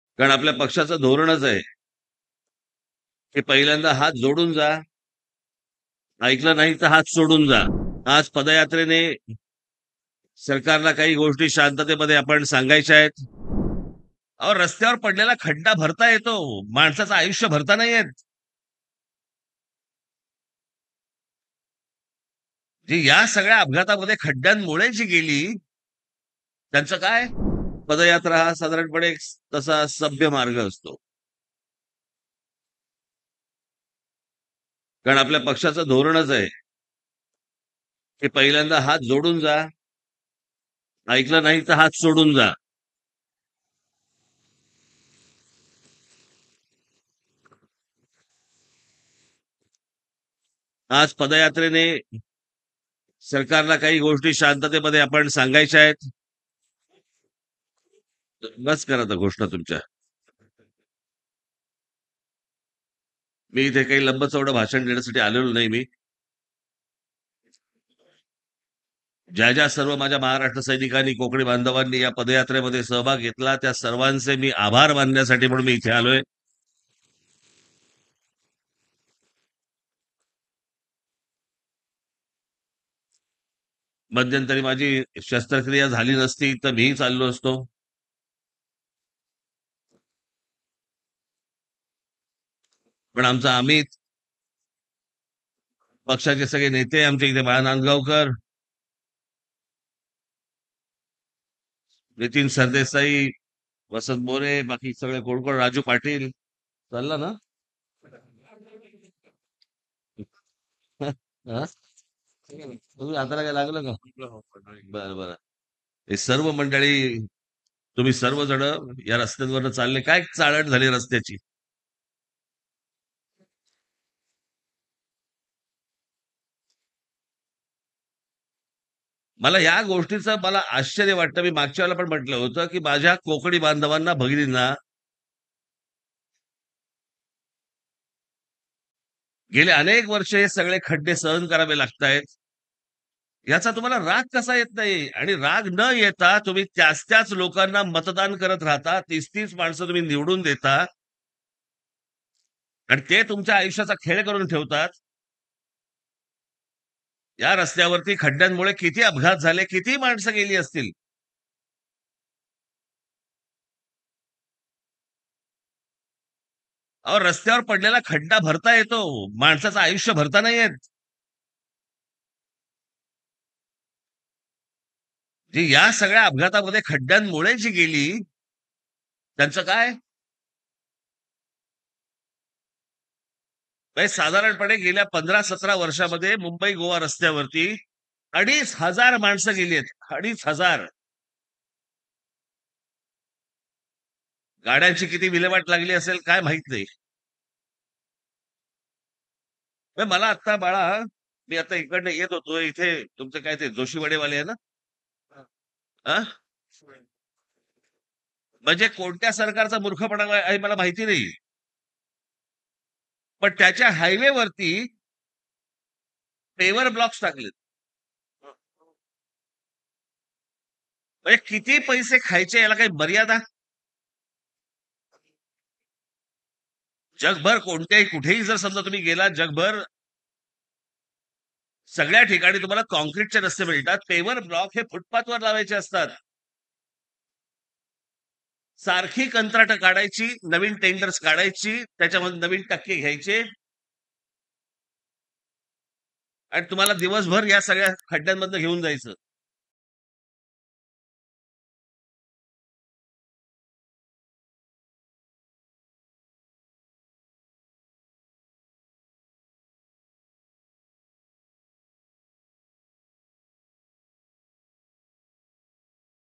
Raj Thackeray : रस्त्यावर पडलेला खड्डा भरता येतो, माणसाचं आयुष्य भरता येत नाही | LetsUpp Marathi
रायगड जिल्ह्यातील कोलाडमध्ये मनसे अध्यक्ष राज ठाकरे यांनी कोकणामधील रस्त्यांवरुन जोरदार टीका केली. यावेळी गेल्या 15 वर्षात मुंबई-गोवा महामार्गावर अपघातामुळं अडीच हजार जणांचा मृत्यू झाल्याचे राज ठाकरे म्हणाले.